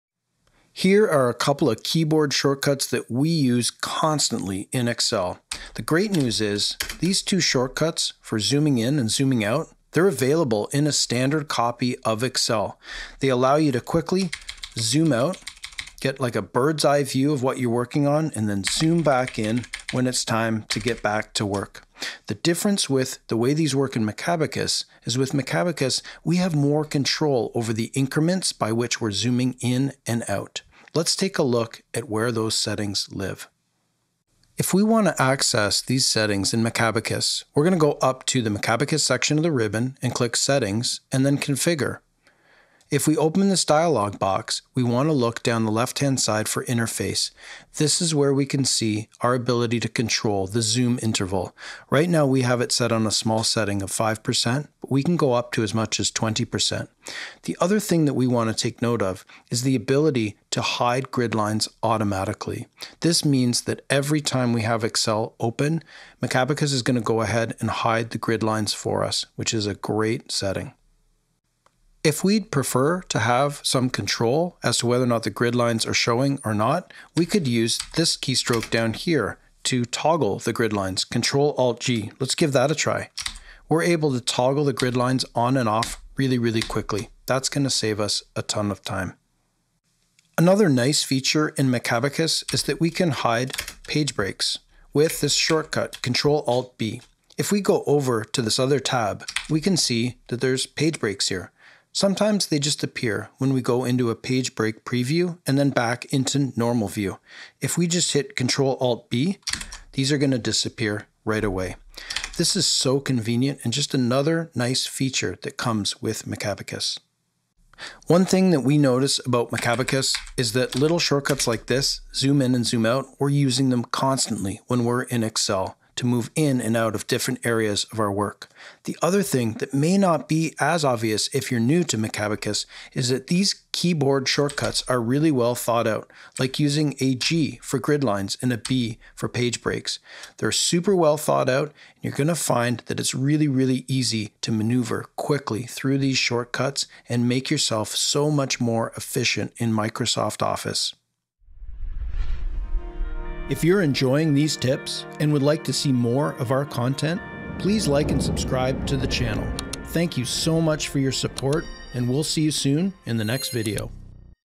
Excel | View & Interface Part 1 | Macabacus
Use Zoom In and Zoom Out to adjust zoom levels in selected worksheets by a specified interval. Repeat the keystroke to continue zooming in or out. The zoom interval can be set to 5%, 10%, 15%, or 20% in Macabacus. Use Toggle Gridlines to turn gridlines on or off. Hide Page Breaks can be used to turn off page breaks on selected worksheets when in Normal View.